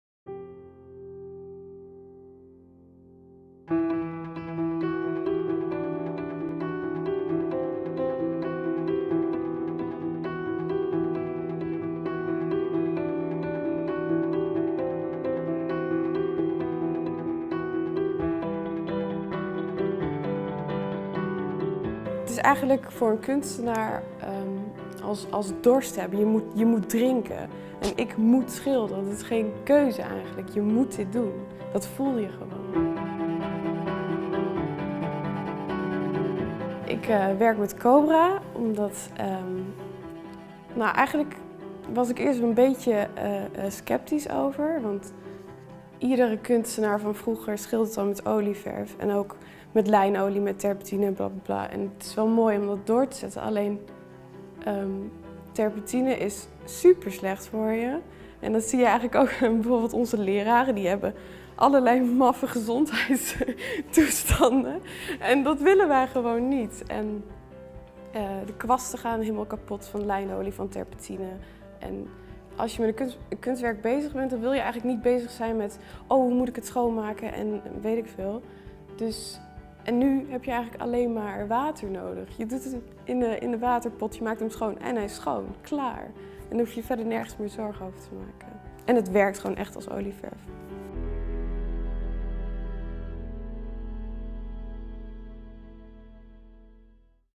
Cobra Artist review - Why use water mixable oil paint?
In this video a Cobra artist reviews the usage of water mixable oil paint. Why does she use Cobra oil paint? What is the quality of water mixable oils? Why do other people use water mixable oil paint? In this oil paint review she will explain.

Why does Cobra artist Merel paint with water mixable oils?
At first, Cobra artist Merel was a little bit sceptical about painting with Cobra. About all painters from history painted with oil, turpentine, linseed, spirit and so on. She also wanted to continue painting with oil paint. However, turpentine is really bad for people. People even get some weird illnesses from it. That’s not what she wanted. Therefore, she started painting with Cobra oil paint. The second reason for using Cobra’s oil paint is that the brushes are easy to clean. With traditional oil, most of the time you throw your brushes away.

What is the quality of water mixable oils?
The quality of Cobra paint is great. It consists of pure oil. Mixing this oil with water ensures that the oil keeps its colour. Try it yourself!

Why do users paint with Cobra?
Our users paint with Cobra for all sorts of reasons. First of all, due to the fact that you can paint with Cobra without having to use solvents, but also because of the wonderful colours and comprehensive range.

Suggestions
Perhaps we will discuss that subject in one of our newest videos!